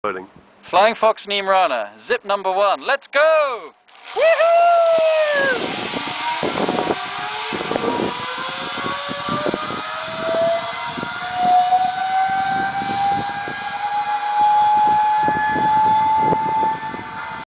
jono's first zip
The founder's first zip at our first zip tour site.